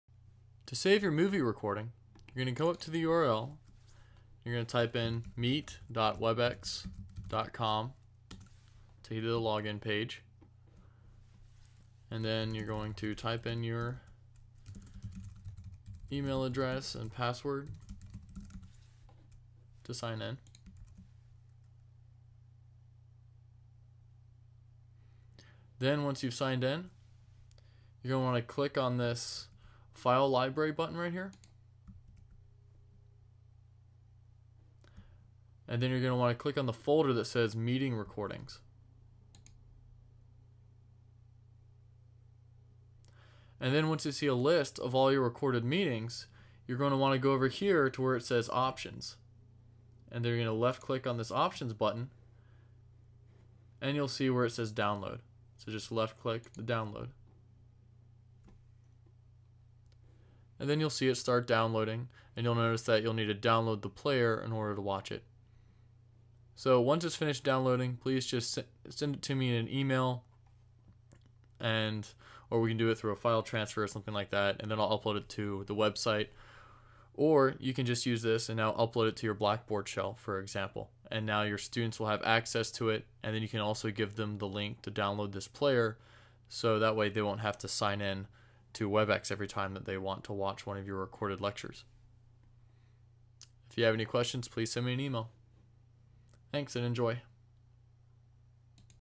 WebEx Recording Download
How to download the Recorded Meeting file.